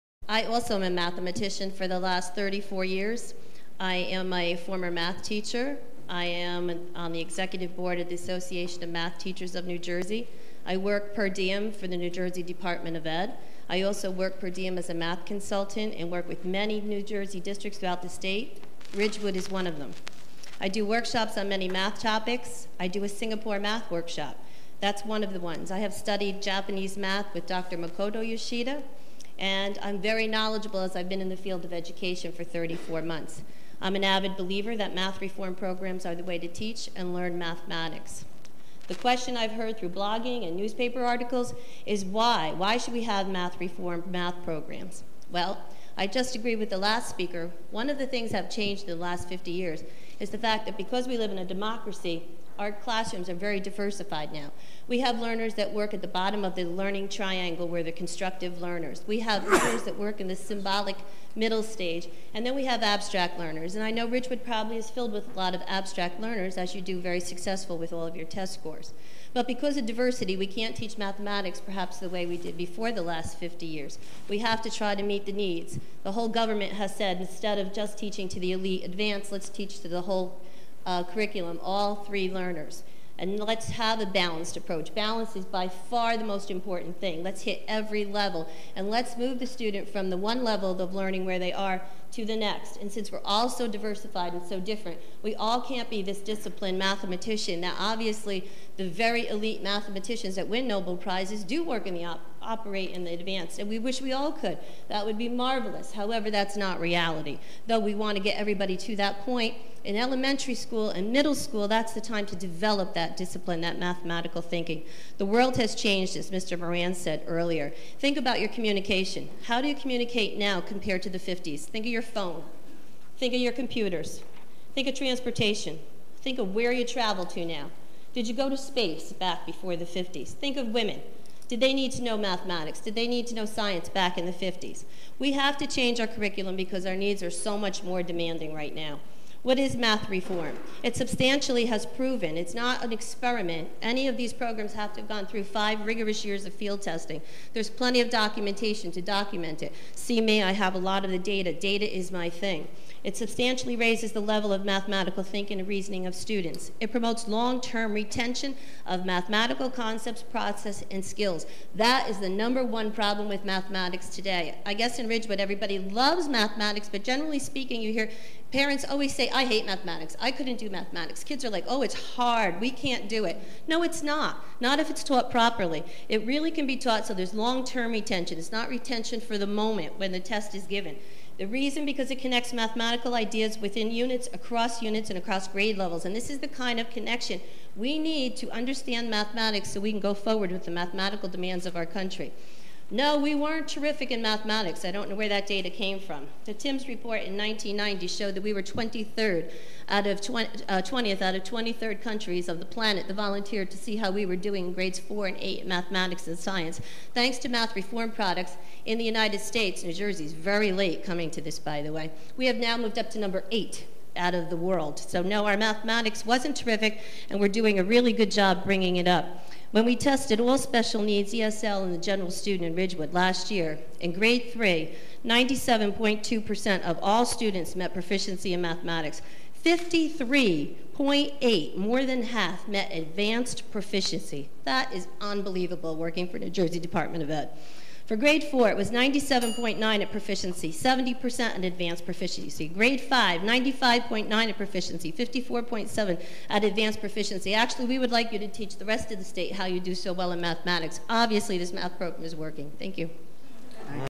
Educator states reform math works!
Citing all those things we've read in articles - professors saying stop sending students who only compute, test scores are wonderful ... all in defense of reform math - made for all. What I would like to know, is this person true views on Singapore math and do they consider that reform math too?